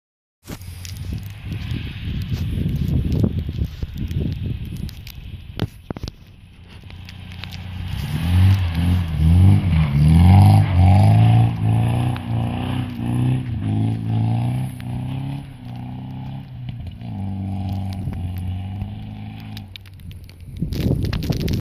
Volvo 144 on Snow!
First snow here in Sweden. snow = Fun!
Enjoy/Foffe